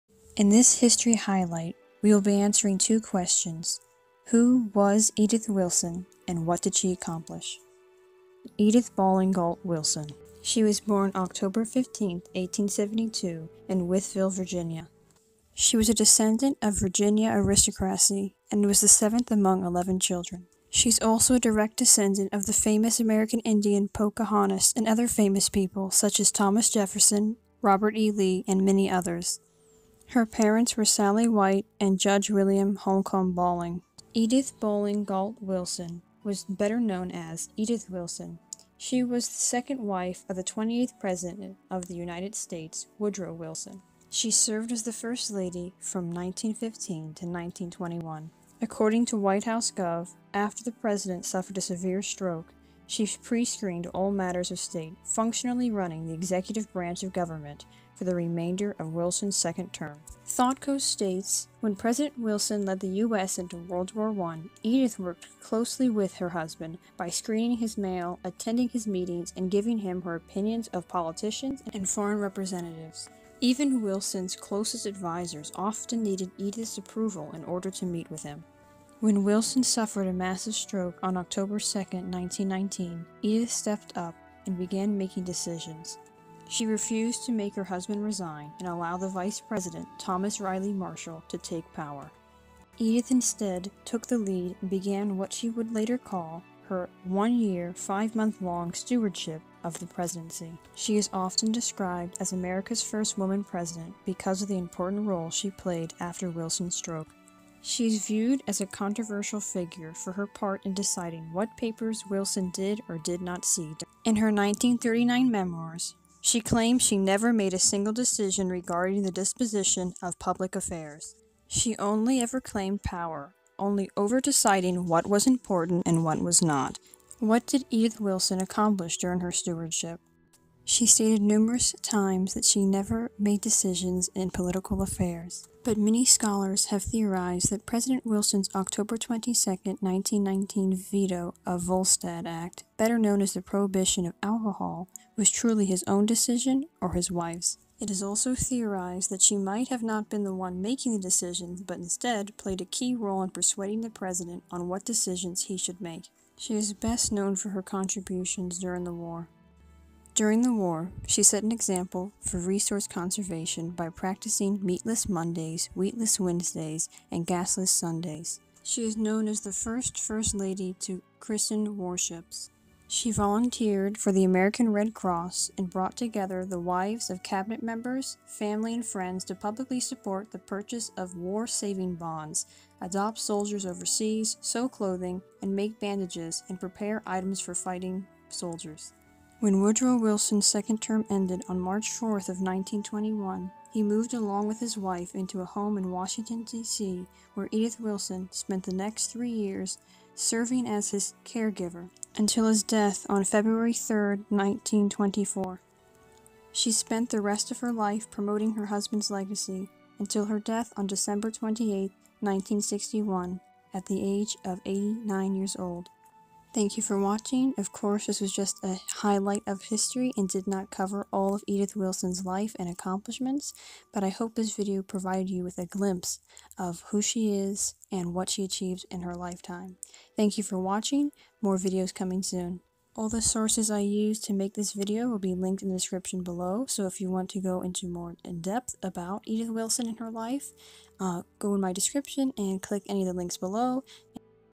History Highlight| Edith Wilson: The First Woman President?!
In this History Highlight we discuss Edith Wilson's secret Presidency.
Videos & Images used in this video do not belong to me.

Additional Sources

Genealogy - Edith Bolling Wilson Museum Wytheville, VA – Explore History in Southwest Virginia

Madam President : the secret presidency of Edith Wilson / William Hazelgrove | Smithsonian Institution